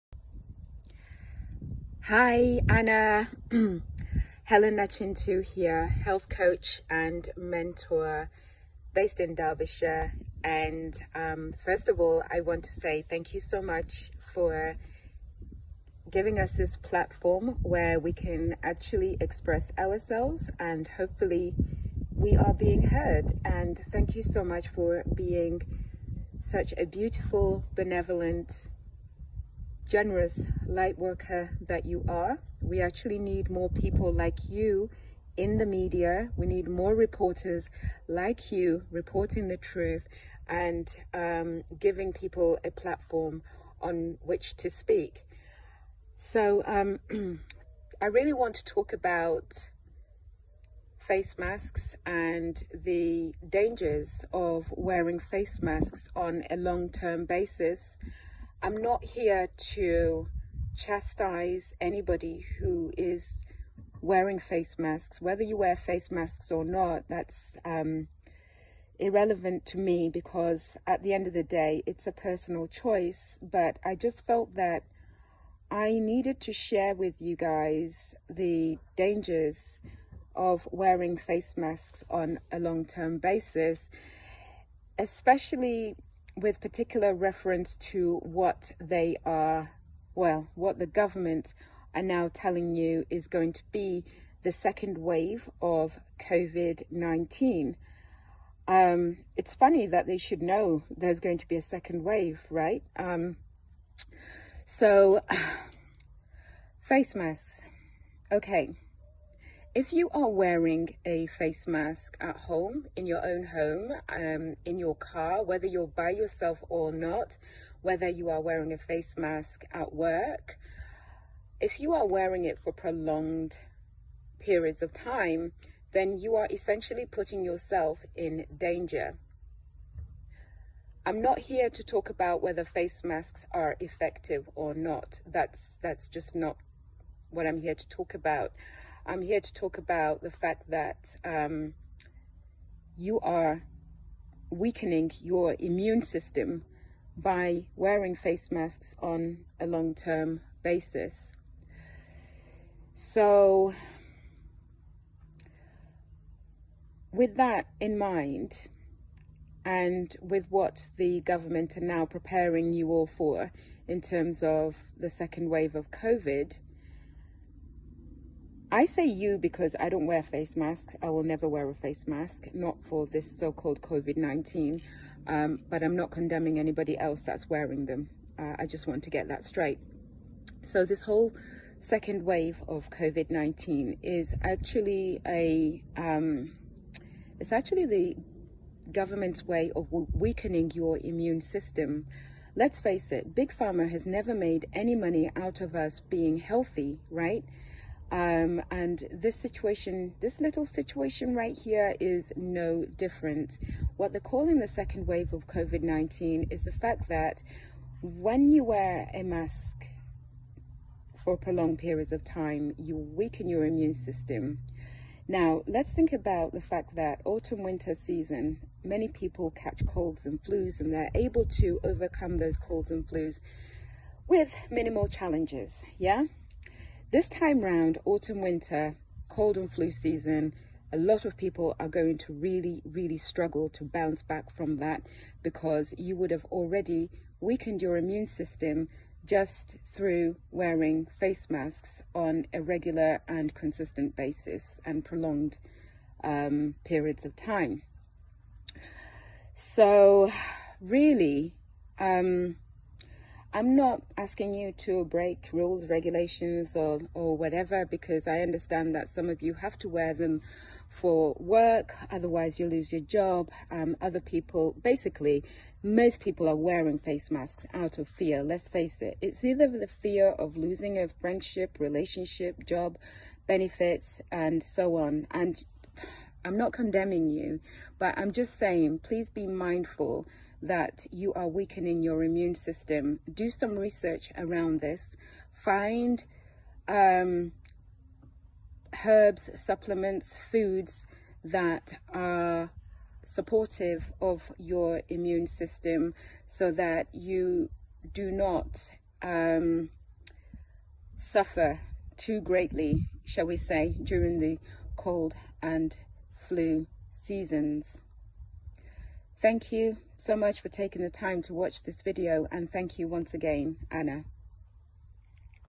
"The Mood of Britain" #102, August 2020 (mirrored for Extinction Rebellion)
Helen Nachintu, 44, Health Coach, Derbyshire  8th August 2020.